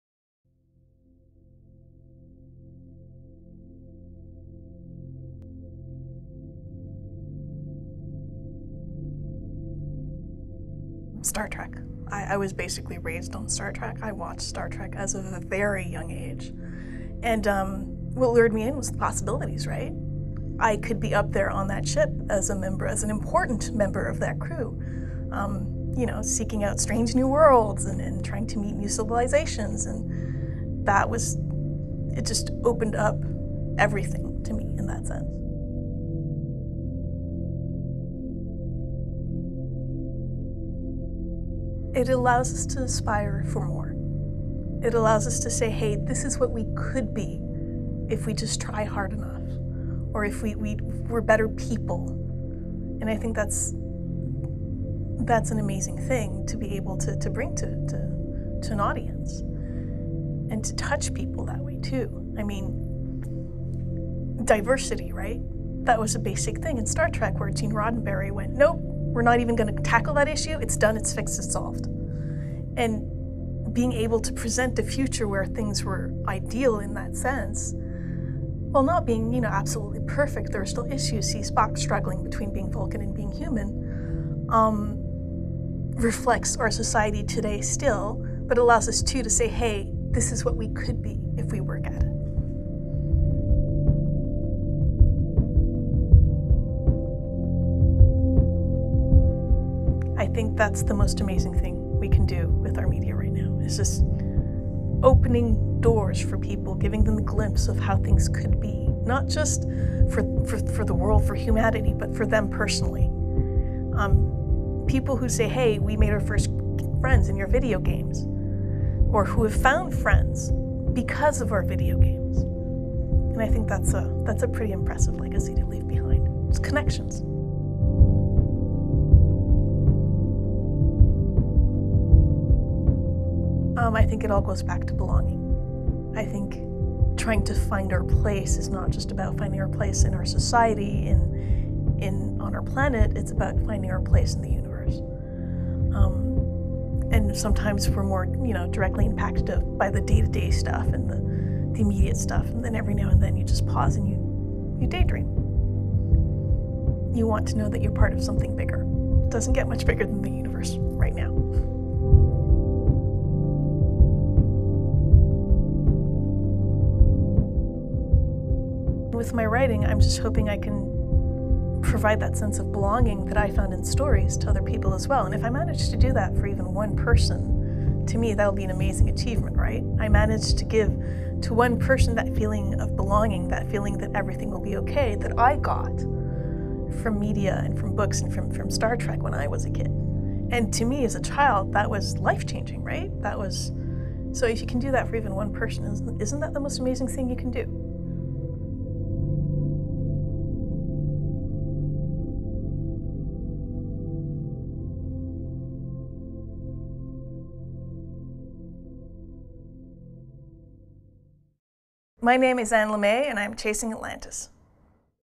Writer Ann Lemay - Finding Belonging in Space: Chasing Atlantis
In an interview with Chasing Atlantis, Ann talks about finding meaning and belonging in space and science fiction and finding her roots in Star Trek. Ann was our favourite interview on the Citadel